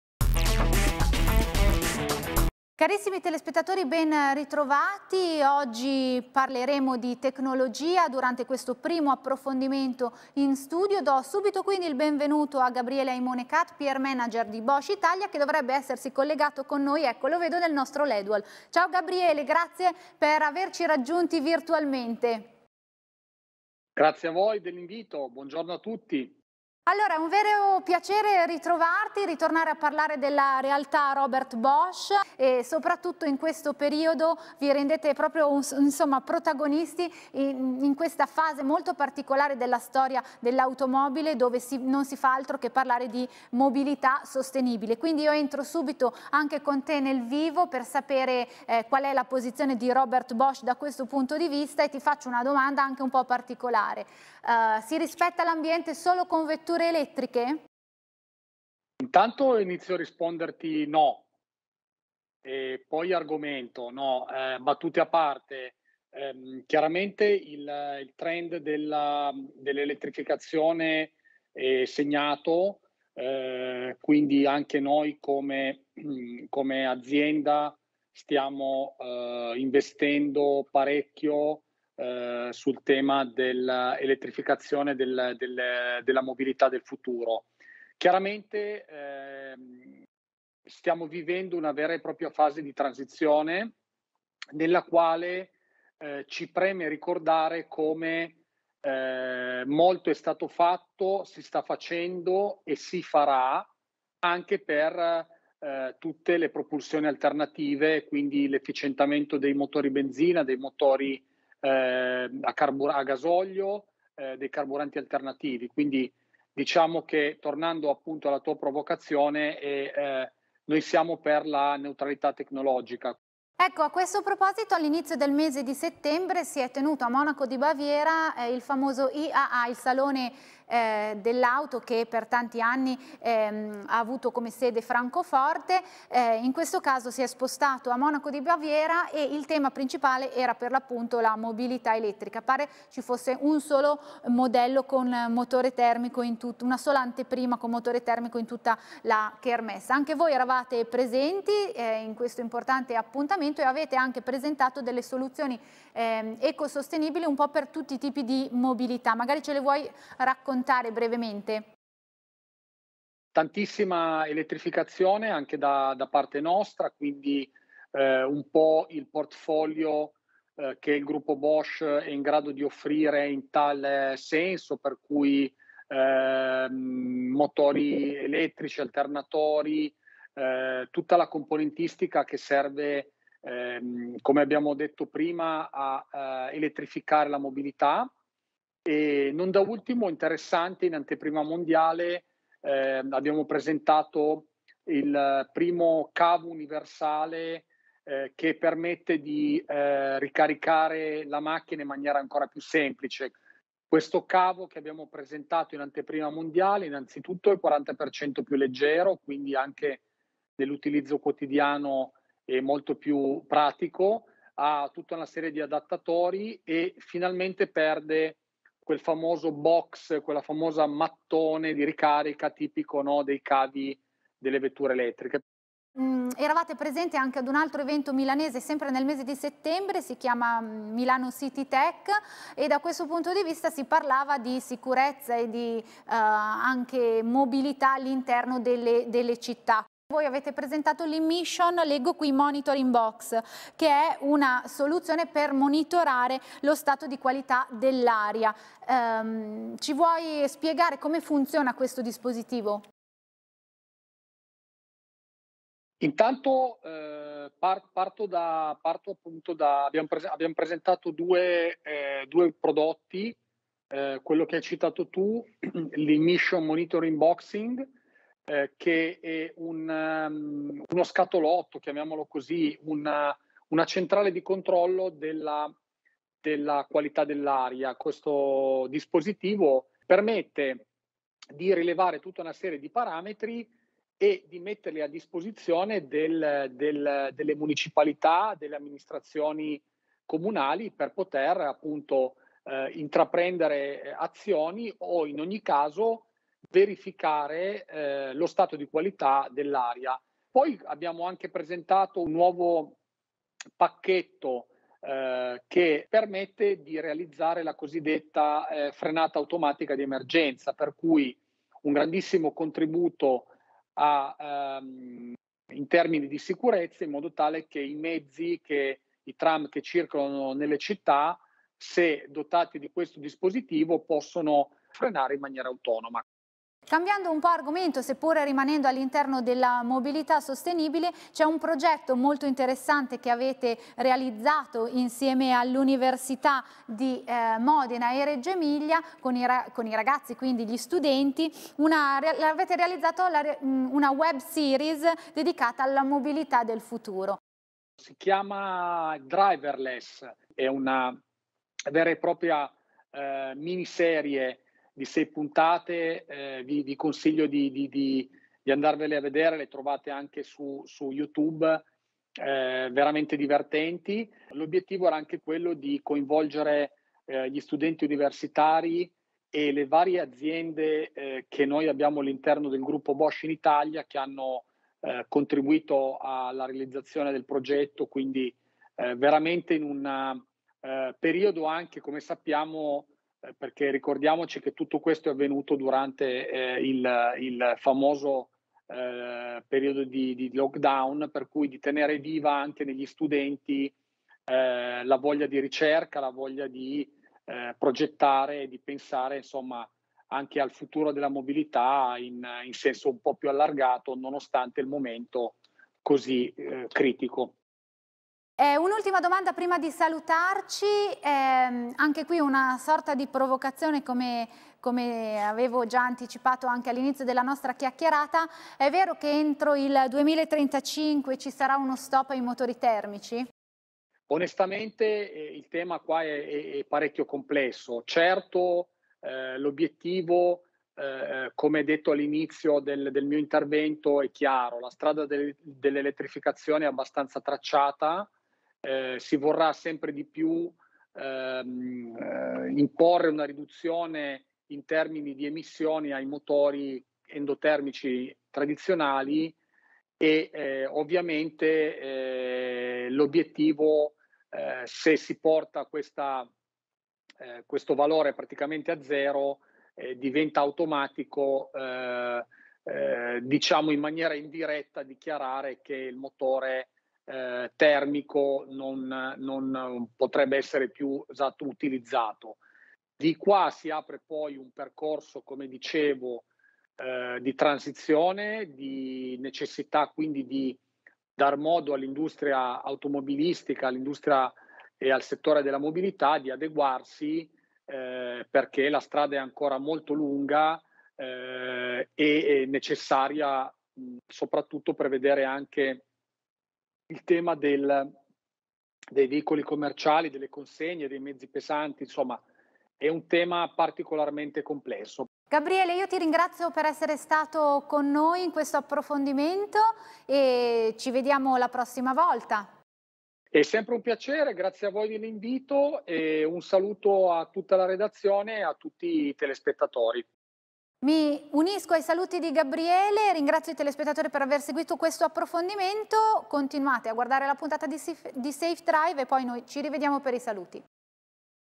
Ospite in Collegamento: Gabriele Aimone Cat Pr Manager Bosch Italia
Durante il primo approfondimento in studio di oggi abbiamo affrontato
l’argomento della mobilità elettrica con uno dei principali
protagonisti del settore. L’azienda Robert Bosch ricopre da sempre un
ruolo fondamentale nella lotta contro le emissioni inquinanti
sviluppando sistemi innovativi in grado di fare la differenza. All’IAA
di Monaco di Baviera, Roberto Bosch ha presentato il primo cavo di
ricarica per auto elettriche senza scatola di controllo,  mentre alla
fiera Milano Citytech ha portato l’Immission Monitoring Box un
dispositivo ideato per misurare la qualità dell’aria nei comuni.